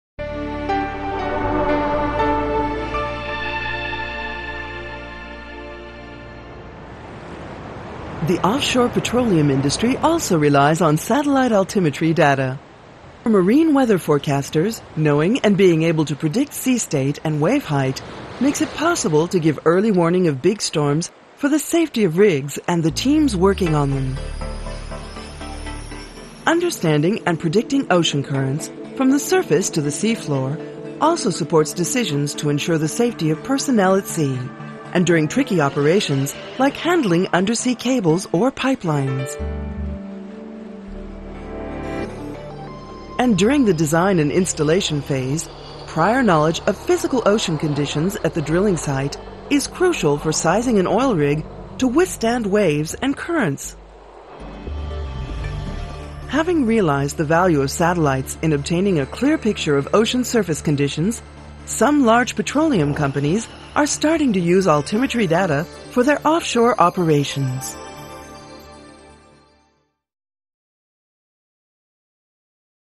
Offshore activities (2008)
Credit Cnes/Mira production 2008
The offshore industry includes all commercial activities that take place at sea, with the exception of fishing and maritime transport. It covers oil platforms and power generation (from winds, currents and waves) as well as the installation of pipe-lines for the former and submarine cables for the latter and also for telecommunications. The ocean is a challenging environment. Winds, waves and currents are sometimes described with menacing adjectives to describe sea states that are potentially hazardous to sailors navigating on the surface, such as squalls, cross seas, adverse and changing currents, breakers or rogue waves.  Reliable sea state forecasts, including wave height and direction and current velocity, are of great benefit to all offshore activities. Satellite altimetry provides systematic, global, homogenous and constant cover and is unaffected by weather conditions (working even at night or through cloud) which makes it a very useful source of input data for forecasting models. Calculating sea height by altimetry helps to identify and measure the currents that swirl around the bumps and hollows on the surface of the oceans. Scientists can use the data to plot global or meso-scale ocean circulation on charts of currents and to measure their variations. Sea height is not the only variable of interest to offshore activities, however, and altimeters can provide other types of valuable information: the amplitude and shape of the radar echo can indicate wind speed and wave height. Data profiles compiled in situ complete these descriptions to give a fuller description of the masses of water at greater depths. The launch of the Jason-2 satellite, on 20 June 2008, make new data available, improving operational systems for ocean forecasts and marine meteorology and thus contributing to the safety of people and property.